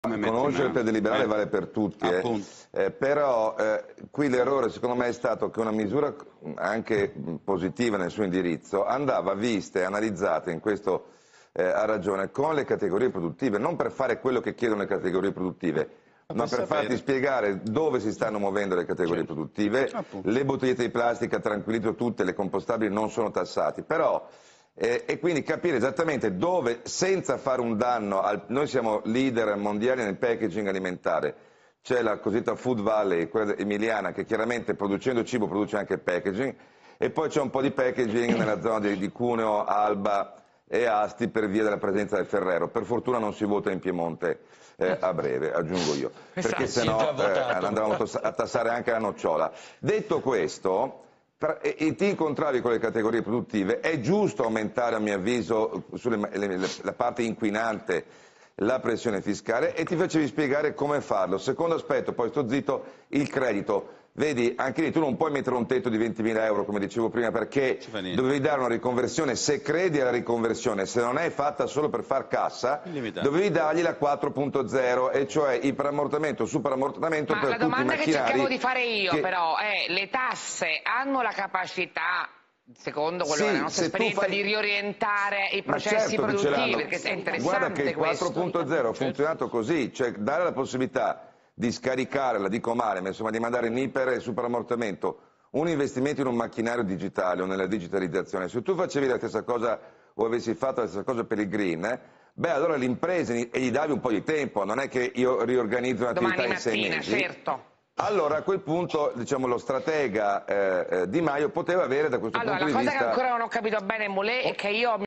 Plastic Tax, Sebastiano Barisoni: "L'errore è stato non analizzare questa misura con le ...
Plastic Tax, Sebastiano Barisoni: "Una misura di questo tipo andava analizzata con le categorie produttive. E' giusto aumentare la pressione fiscale sulla parte inquinante"